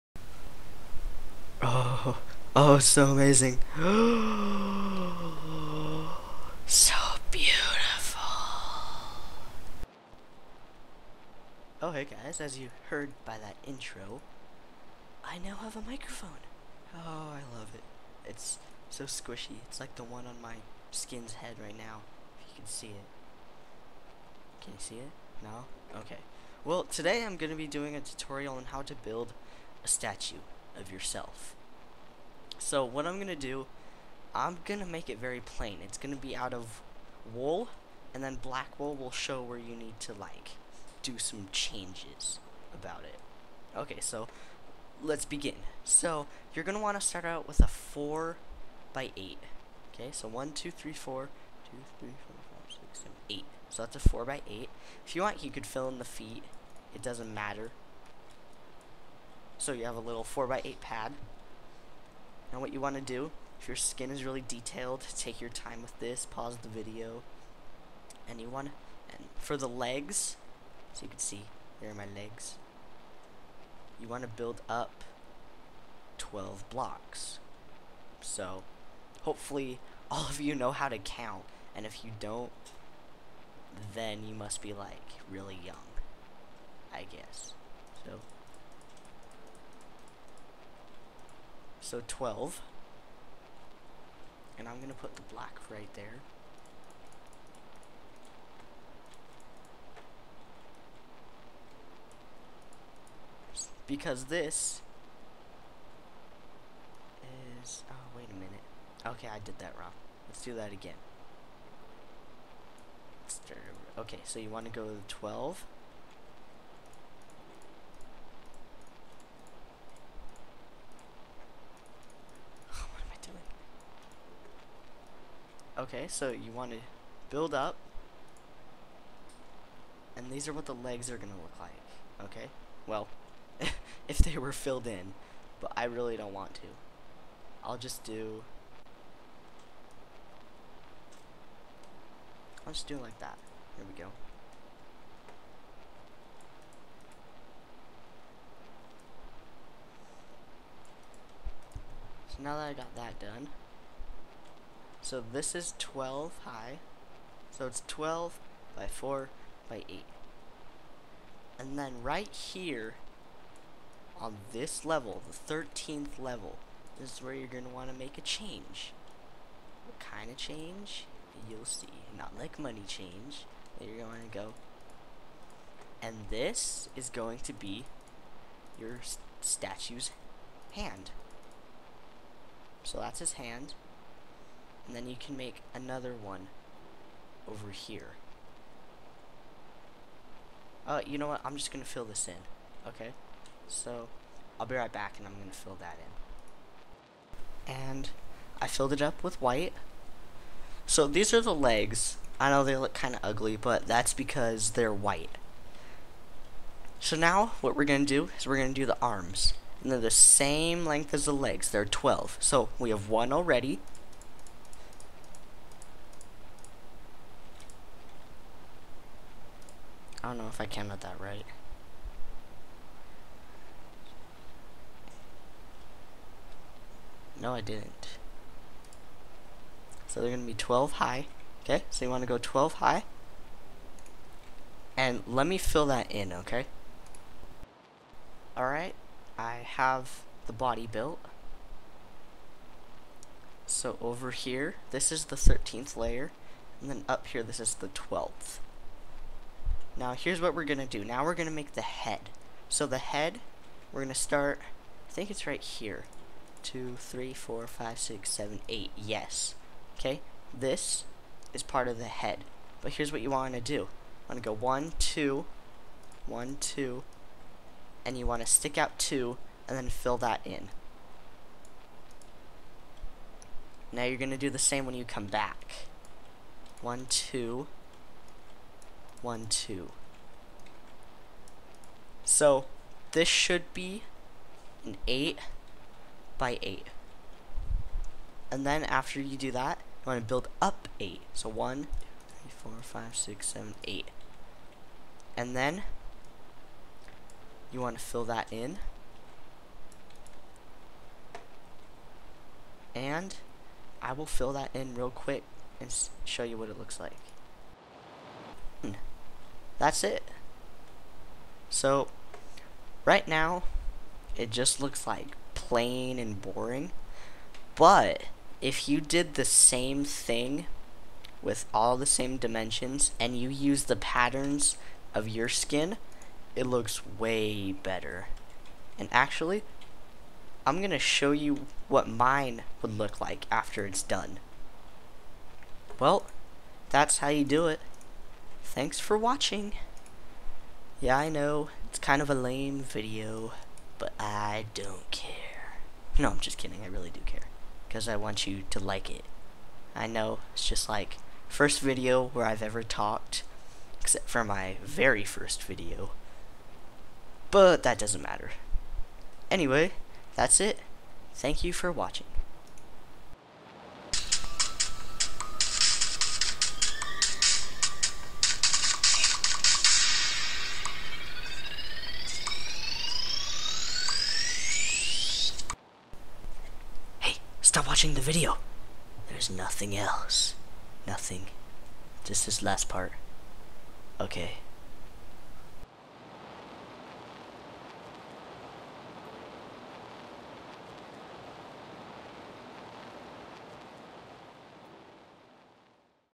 Statue Build Tutorial
In this tutorial, I will show you how to make a statue of your skin. My special secret surprise definitely isn't me talking... Okay, maybe it is...